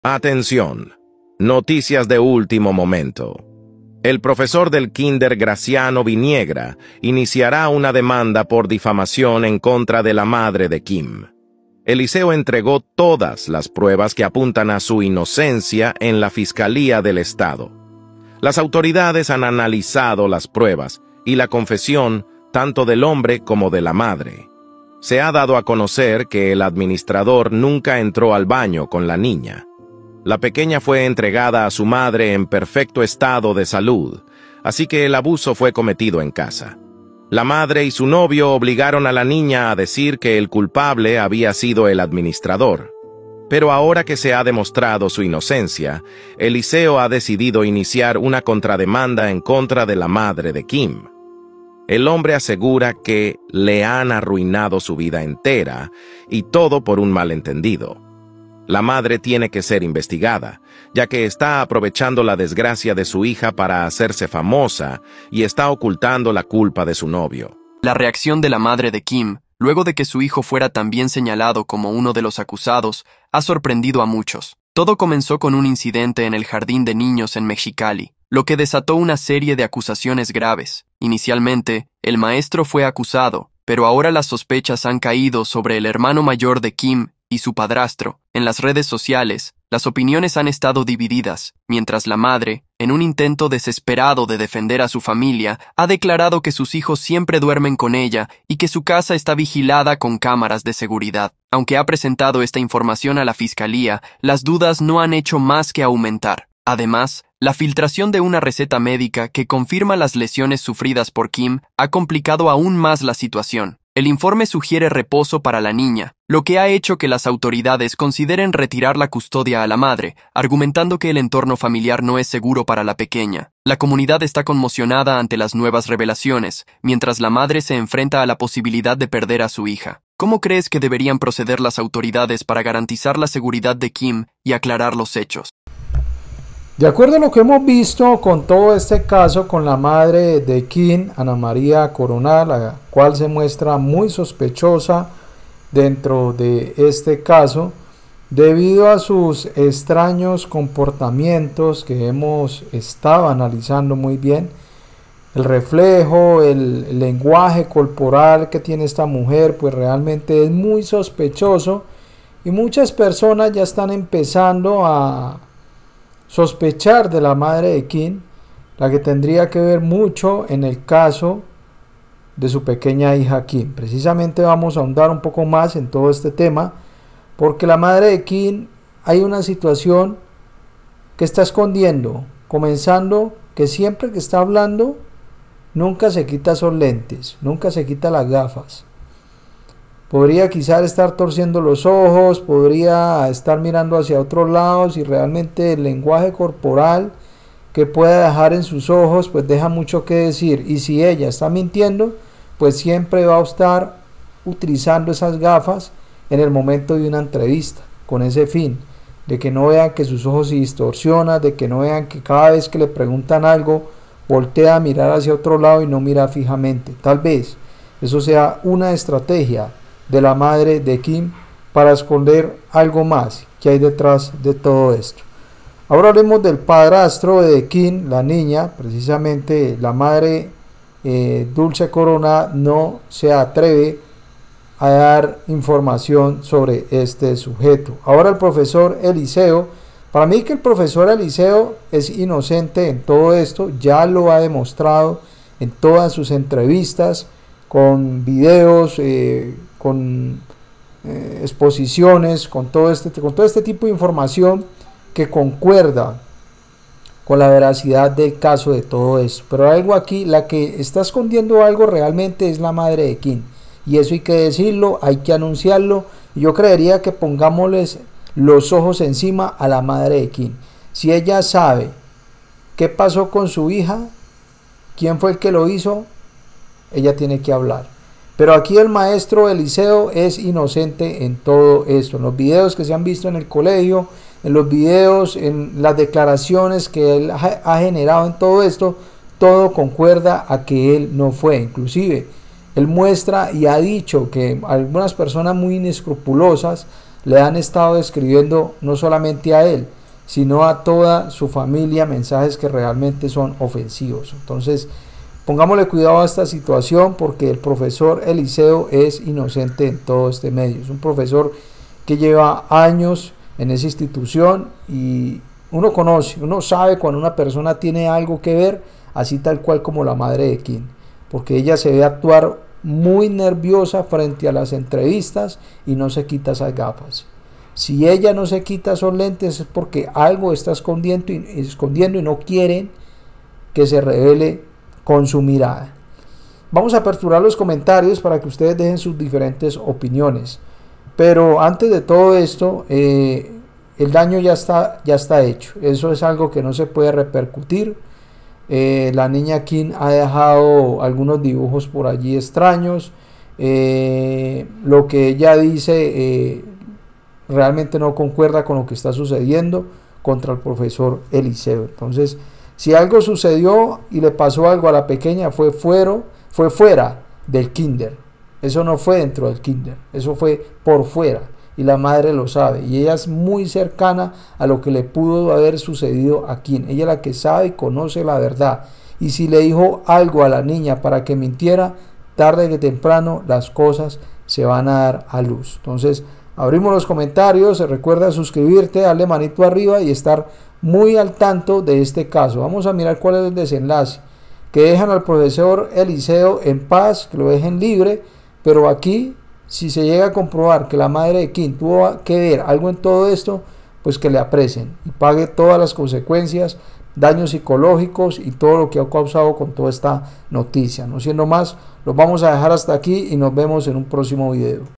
🚨ULTIMA HORA ! Aparece el Culpable Madre de KIM Lo oculto todo, Caso Kim Kinder de mexicali Hoy !
🚨ULTIMA HORA ! Aparece el Culpable Madre de KIM Lo oculto todo, Caso Kim del Kinder de mexicali Hoy !